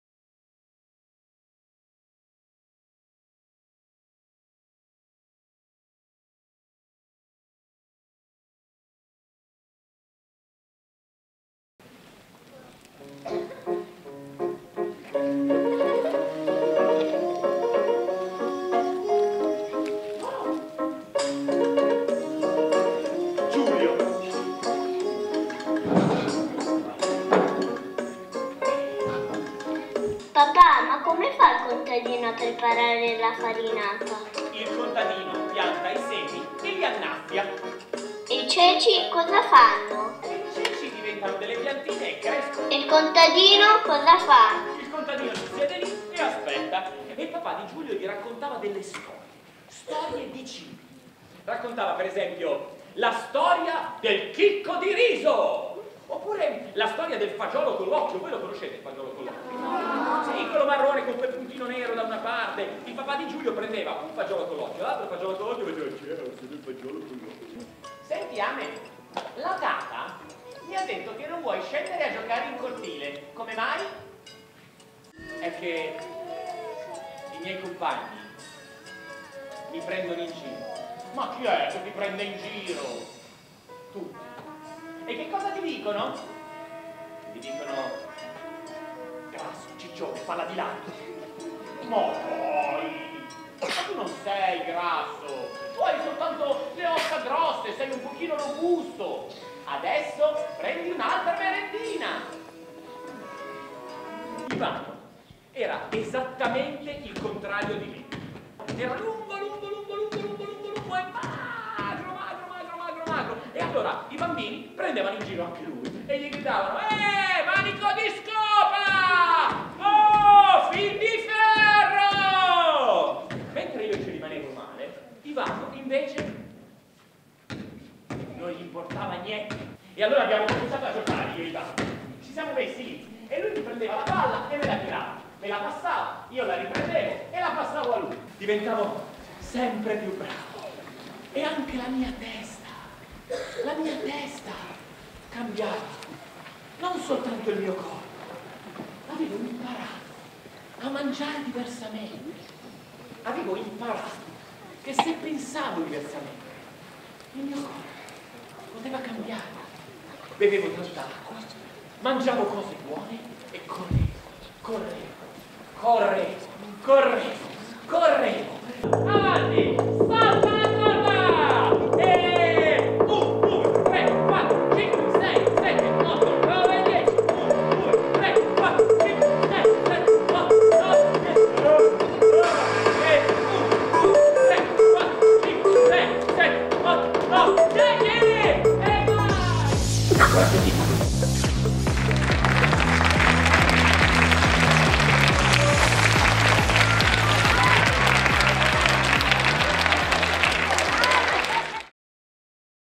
Per Mangiarti meglio demo
Per mangiarti meglio!
Spettacolo per bambini delle elementari sulla prevenzione dell'obesità.
testo e regia Paolo Trotti
con Amedeo Romeo e Diego Paul Galtieri
voce off Olmo Trotti
immagini Paolo Trotti

Riprese effettuate al Teatro Binario7 (Monza)